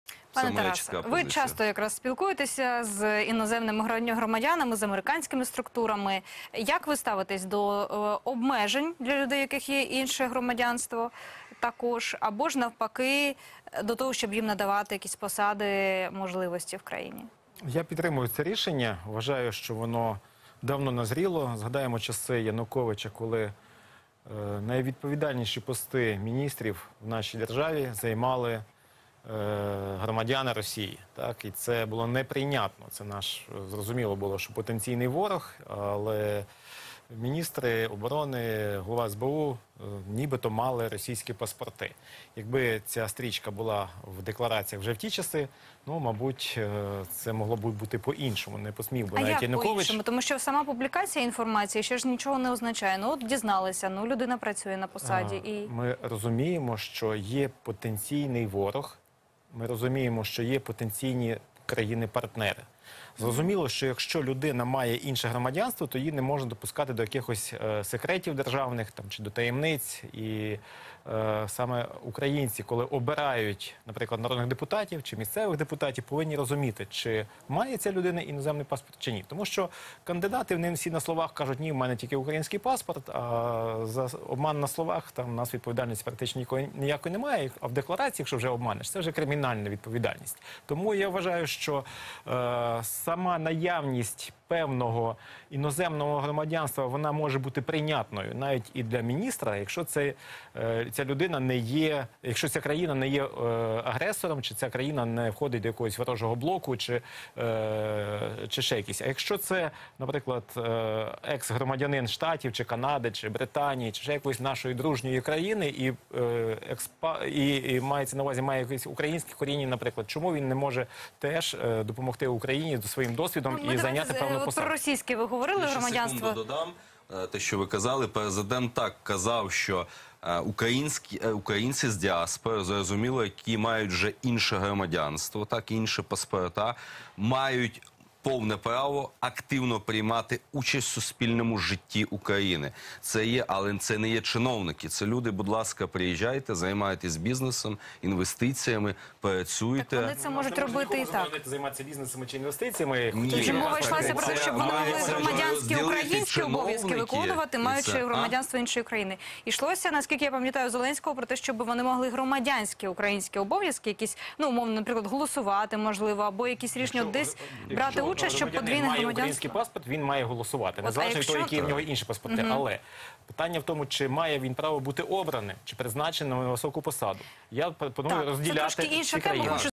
Якщо людина має інше громадянство, то її не можна допускати до державних таємниць, - Козак
Тарас Козак про декларування подвійного громадянства для чиновників